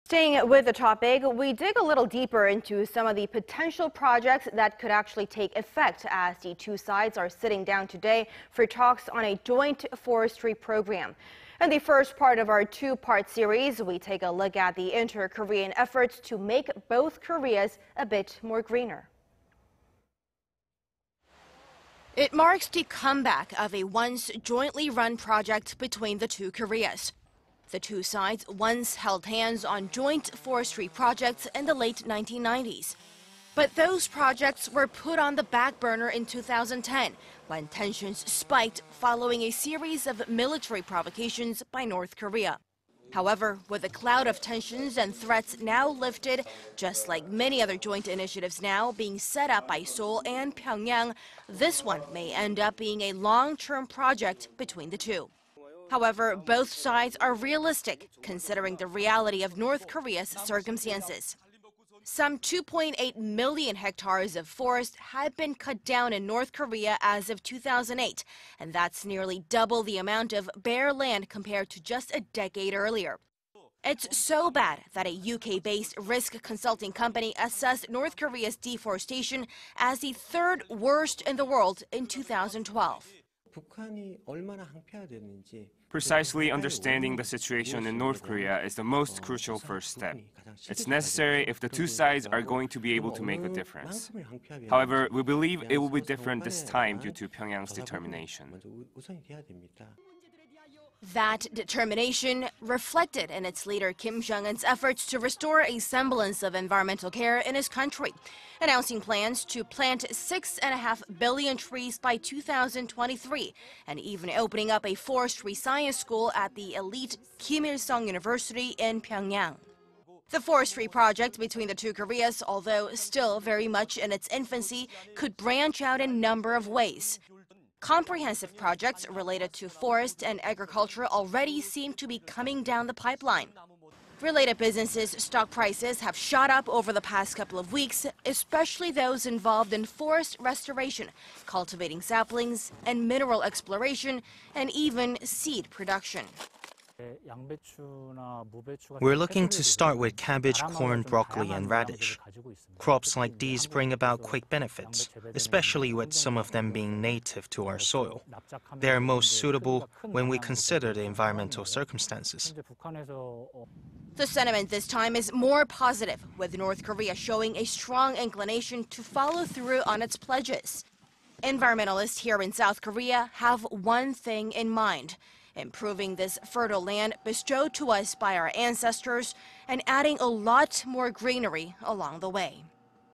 Inter-Korean efforts to make Koreas green again
남북 산림협력, 추진 가능한 방향은?
  Now we dig a little deeper into some of the potential projects that could actually take effect,… as the two sides are sit down today for talks on a joint forestry program.
In the first part of our two part series,... we take a look at the inter-Korean efforts to make both Koreas a little bit greener.

  It marks the comeback of a once-jointly run project between the two Koreas.
  The two sides once held hands on joint forestry projects in the late 1990s.
But those projects were put on the backburner in 2010 when tensions spiked following a series of military provocations by North Korea.
However, with the cloud of tensions and threats lifted,... just like many other joint initiatives now being set up by Seoul and Pyongyang,... this one may end up being a long-term project between the two.
  However, both sides are realistic, considering the reality of North Korea's circumstances.
Some two-point-eight million hectares of forest had been cut down in North Korea as of 2008,… and that's nearly double the amount of bare land compared to just a decade earlier.
  It's so bad that a UK-based risk consulting company assessed North Korea's deforestation as the third worst in the world in 2012.

    "Precisely understanding the situation in North Korea is the most crucial first step. It's necessary if the two sides are going to be able to make a difference. However, we believe it will be different this time due to Pyongyang's determination."

  That determination,… reflected in its leader Kim Jong-un's efforts to restore a semblance of environmental care in his country: announcing plan to plant 6-and-a-half billion trees by 2023and even opening up a forestry science school at the elite Kim Il-sung University in Pyongyang.

  The forestry project between the two Koreas, although still very much in its infancy, could branch out in a number of ways.
Comprehensive projects related to forest-and-agriculture already seem to be coming down the pipeline.
Related businesses' stock prices have shot up over the past couple of weeks,... especially those involved in forest restoration, cultivating saplings, mineral exploration,... and seed production.

    "We are looking to start with cabbage, corn, broccoli and radish. Crops like these bring about quick benefits. especially with some of them being native to our soil. They are most suitable when we consider the environmental circumstances."

  The sentiment this time is more positive,... with North Korea showing a strong inclination to follow through on its pledges.
Environmentalists here in South Korea have one thing in mind,... improving this fertile land bestowed to us by our ancestors... and adding a lot more greenery along the way.